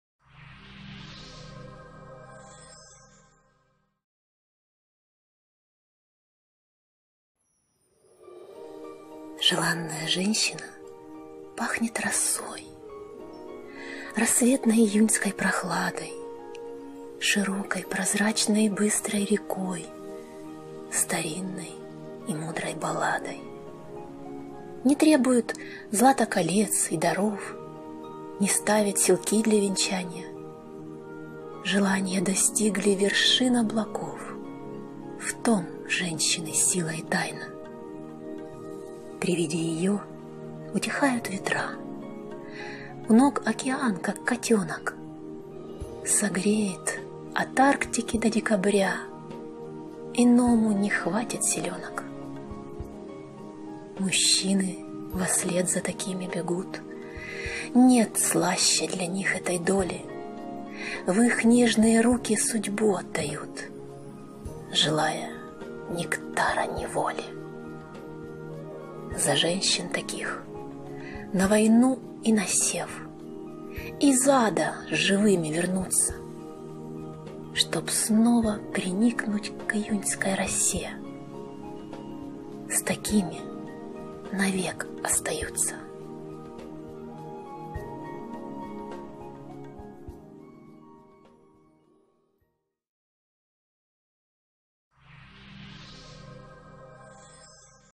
"Желанная женщина пахнет росой" - Виктория Семибратская. Стихи на Конкурс. Июль.
Поэт Виктория Семибратская.
Донецкая Народная Республика, город Донецк.

Читает Алёна Радуга.

Желанная женщина пахнет росой
Рассветной июньской прохладой,
Широкой, прозрачной и быстрой рекой,
Старинной и мудрой балладой.

Не требует злата колец и даров,
Не ставит силки для венчанья.
Желанья достигли вершин облаков,
В том женщины сила и тайна.

При виде её утихают ветра,
У ног океан, как котёнок.
Согреет от Арктики до декабря,
Иному не хватит силёнок.

Мужчины во след за такими бегут,
Нет слаще для них этой доли.
В их нежные руки судьбу отдают,
Желая нектара неволи.

За женщин таких - на войну, и на сев.
Из ада живыми вернутся,
Чтоб снова приникнуть к июньской росе,
С такими навек остаются.